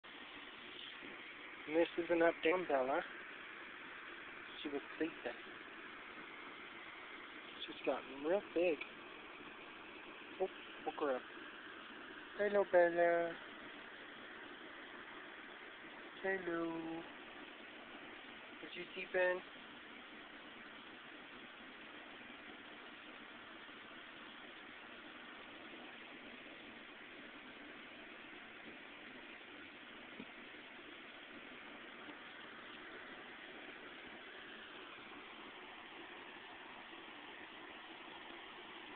Update on my sleeping puppy!!!!
Bella is now a little over a year old and still doing great.